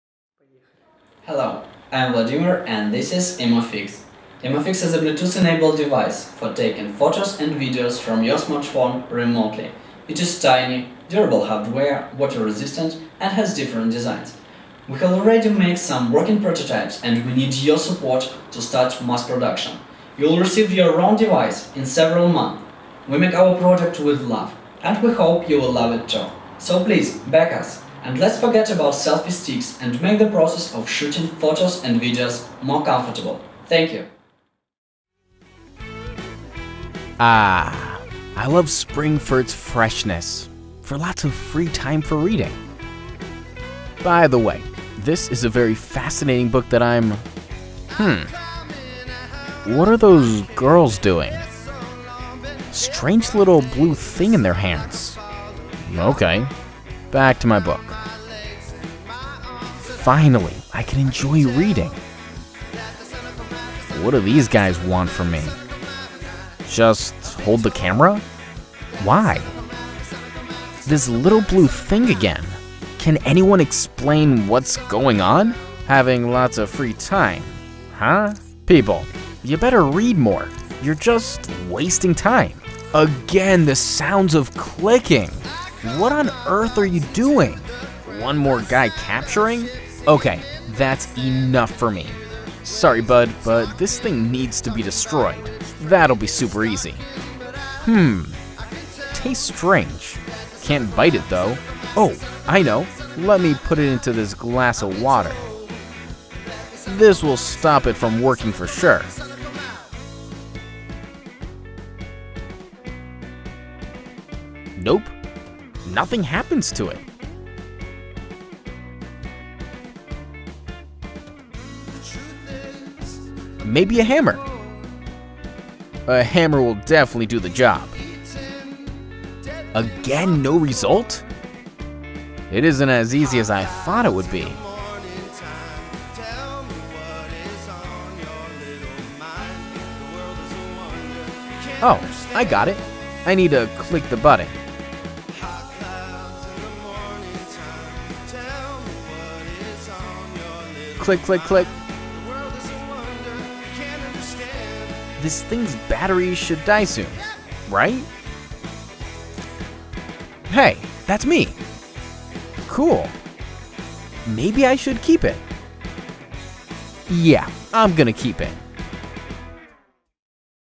Take a Good Selfie with Best Selfie Remote, EmoFix
Let's make the process of shooting video and photos more comfortable. Take a good selfie with the best selfie remote, EmoFix. Share our campaign with your friends and family from your Facebook, Twitter, or Tumblr account to support and encourage us in order to make our Bluetooth launching campaign successful.

It will really help us.